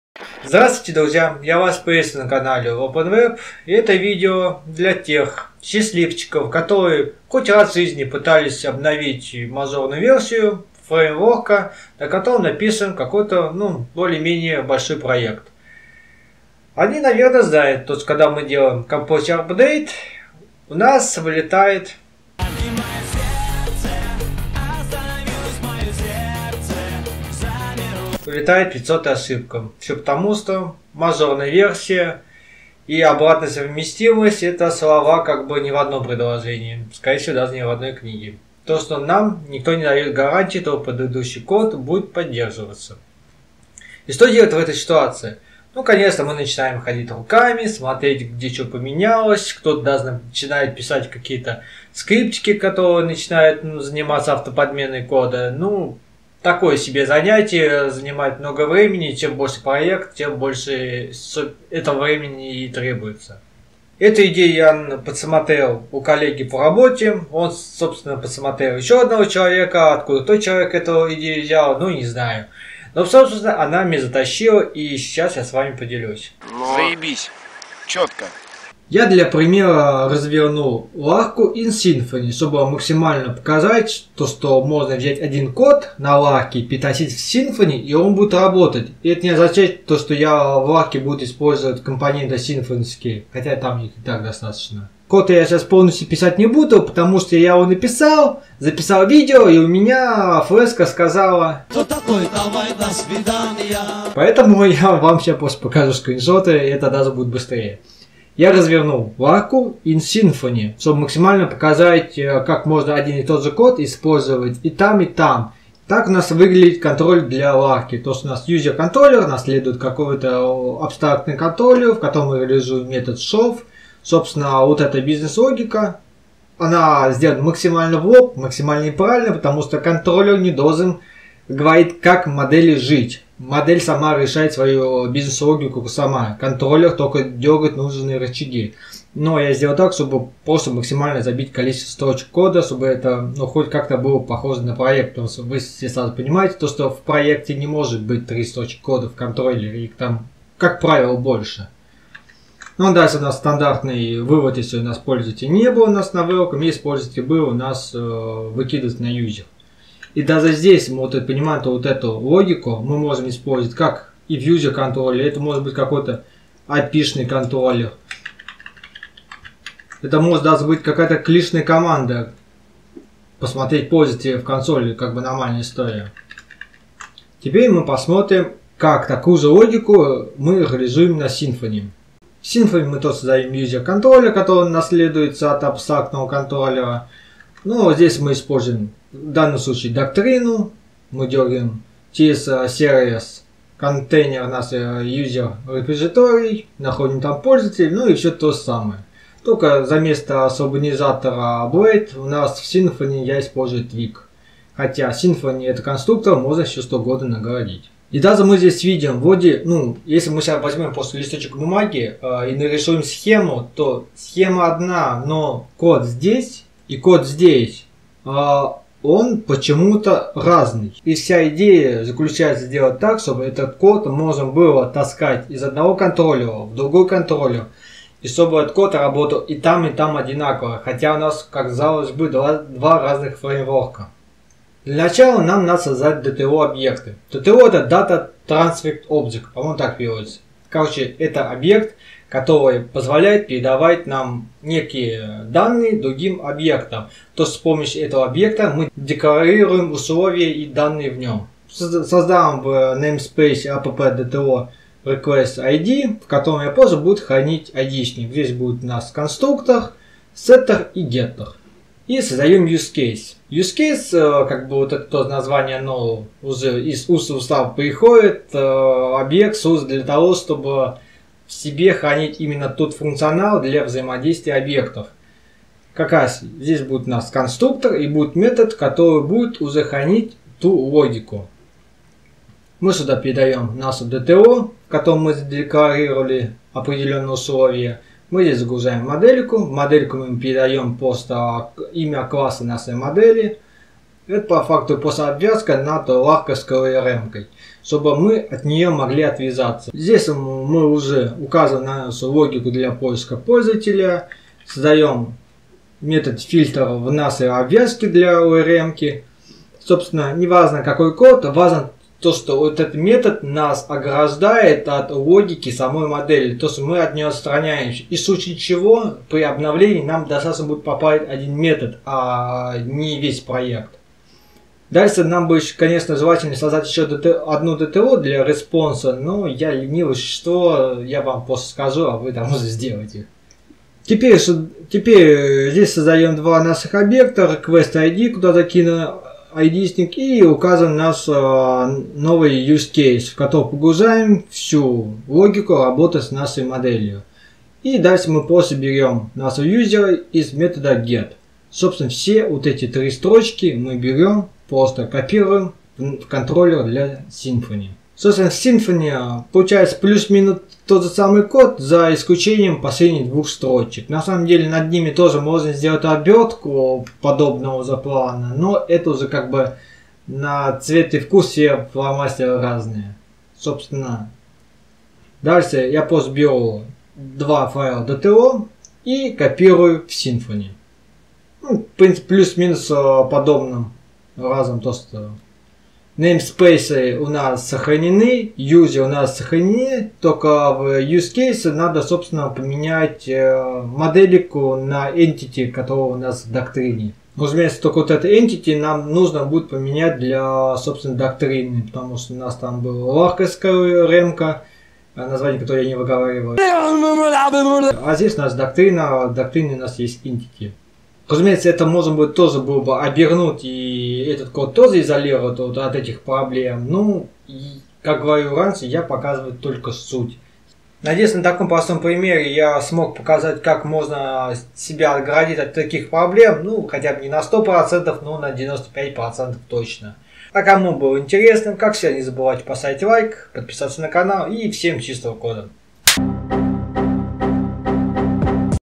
Как сделать код проекта независимым от фреймворка и библиотеки
В видео покажу одни из паттернов которые позволят сделать ваш код независимым от версии фреймворка и библиотек